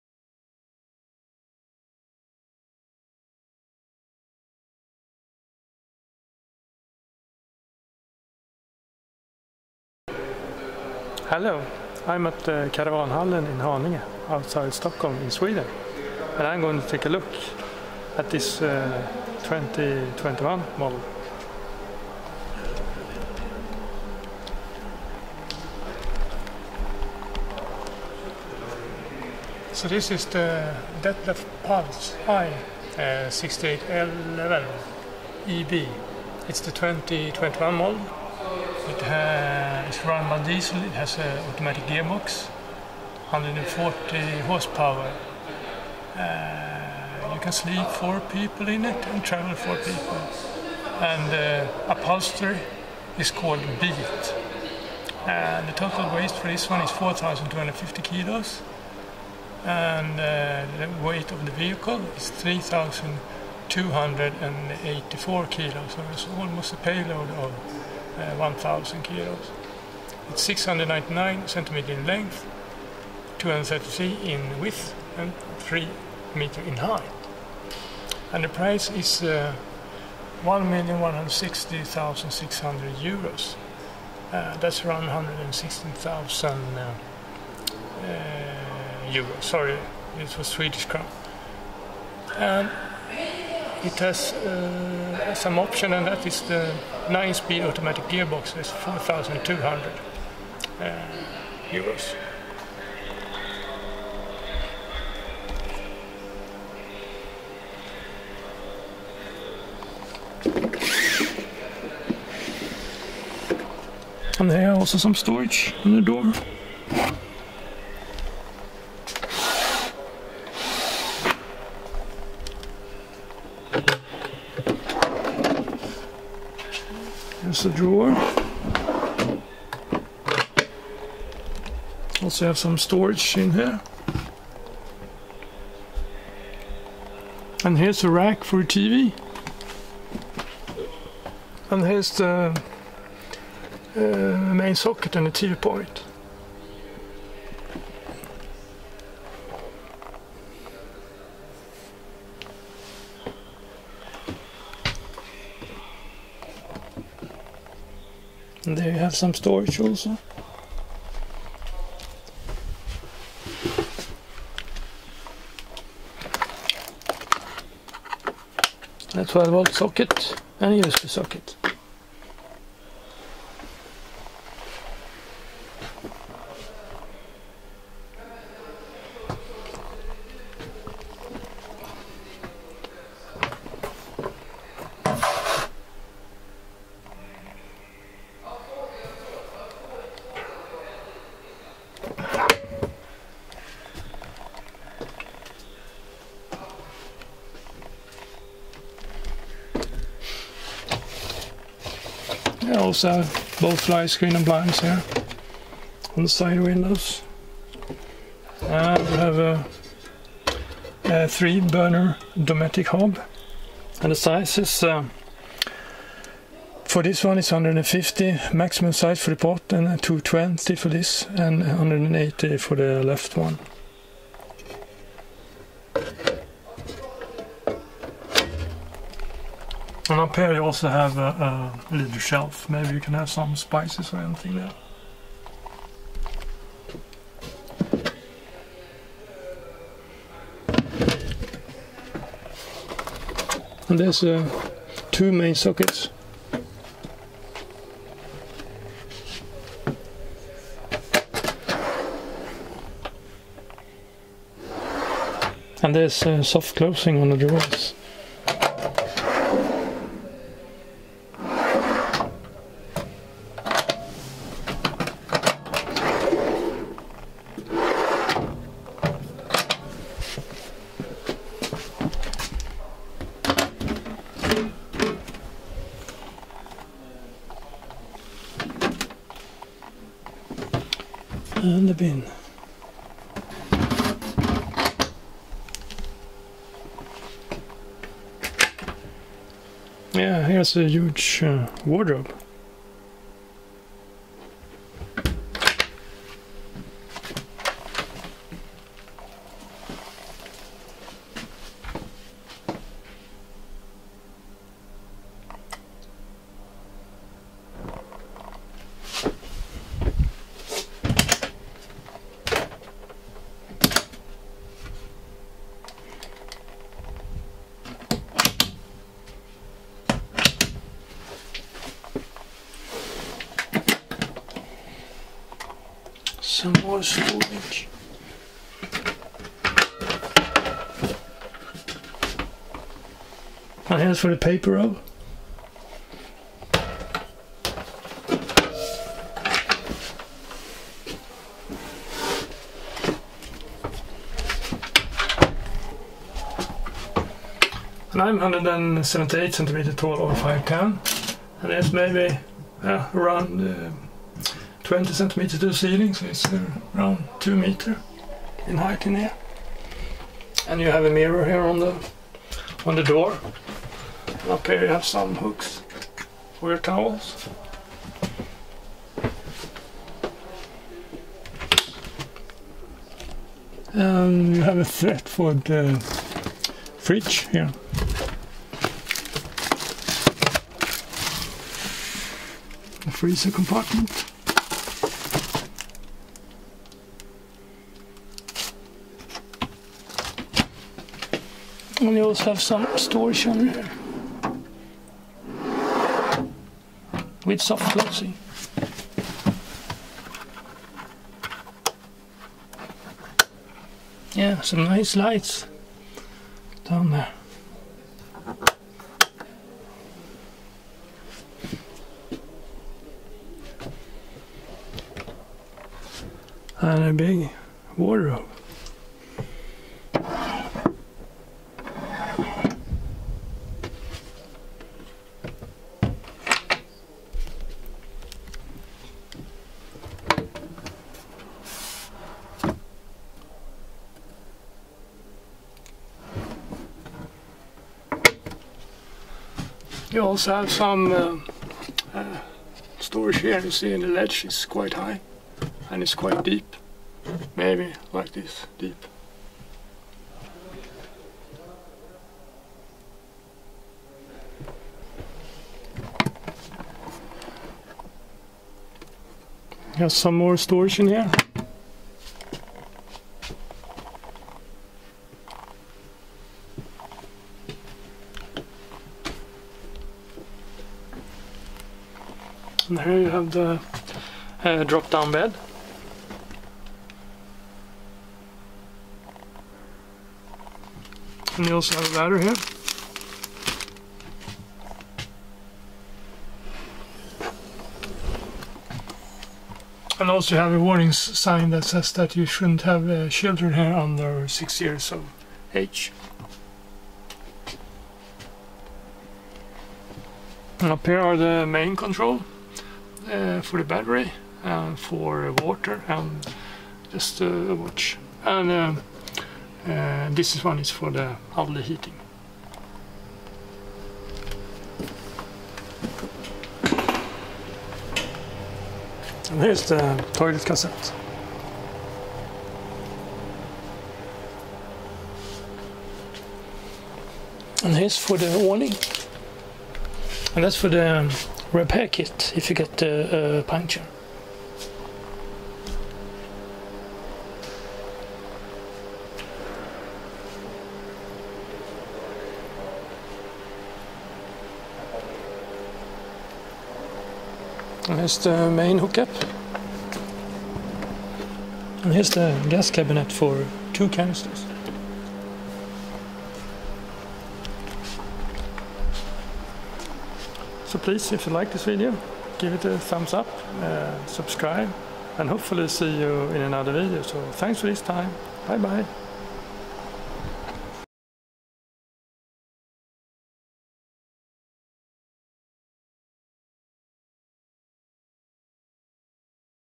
Dethleffs Pulse I 6811 EB Master Class 2 2021 Motorhome 6,99 m
First look of Dethleffs I 6811 EB Master Class 2 2021 motorhome at Caravanhallen in  Haninge in Sweden. This motorhome is 7,41 cm long, 233 cm wide and 300 cm high. I am 178 cm tall.
Price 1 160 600 SEK ( 116 000 euro)